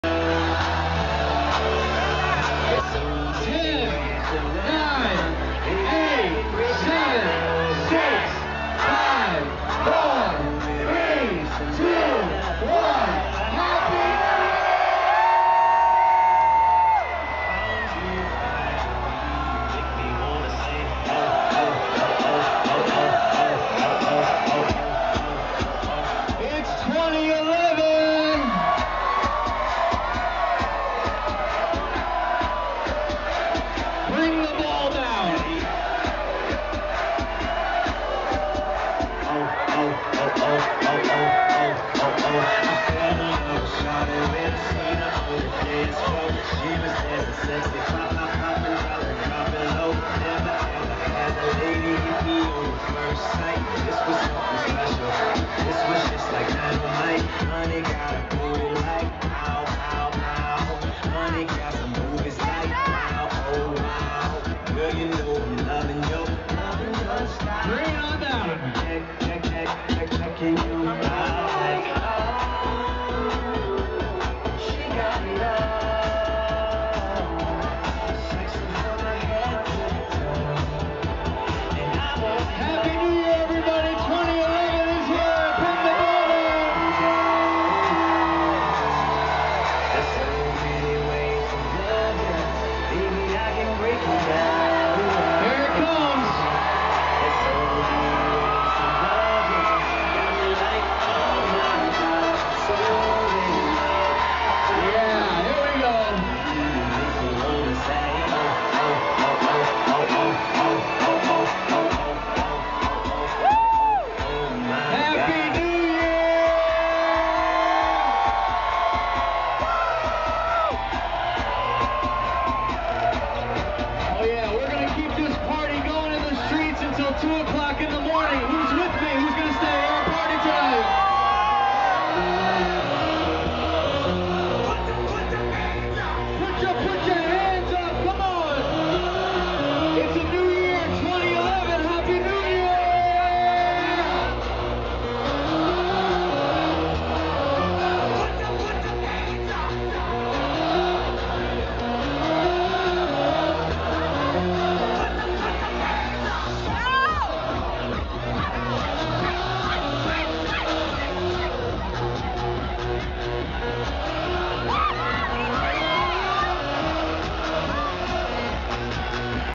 NEW YEARS EVE 2010 DOWNTOWN FT. MYERS
It was a little difficult to get good video of this as I was attempting to help keep everyone from being right under the ball as it dropped...It took a little longer for it to drop than expected, but it was a great night!